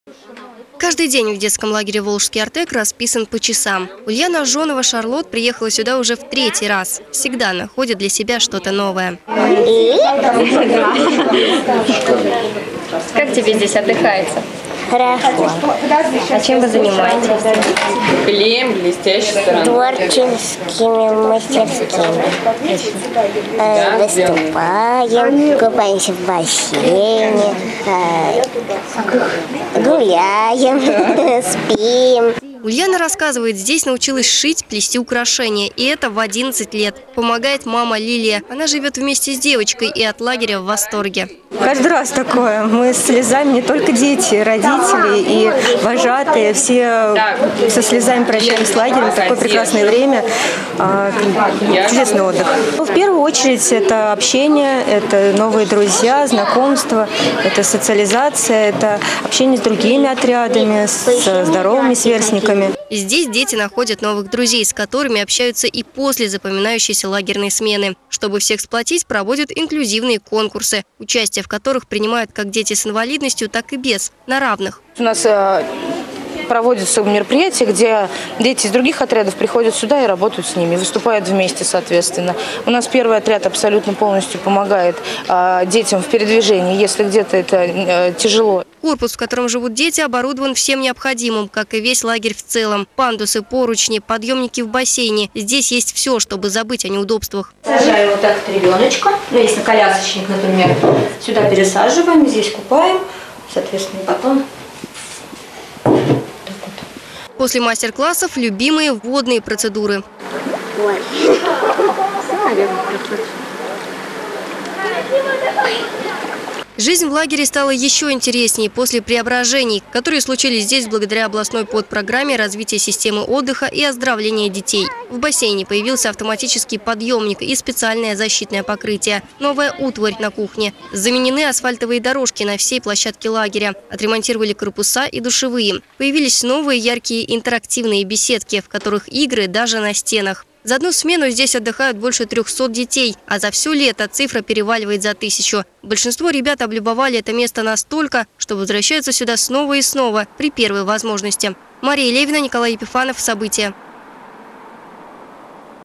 В лагере "Волжский Артек" отдохнули более тысячи детей
Найти новых друзей, и весело провести время. В лагере Волжский Артек каникулы продолжаются. Здесь уже отдохнули больше тысячи детей. За одну смену центр встречает почти 4 сотни ребят. Волжский Артек претерпел изменения, благодаря участию в областной подпрограмме Развитие детского отдыха и оздоровления. Что нового появилось в любимом самарцами центре отдыха узнала Мария Левина.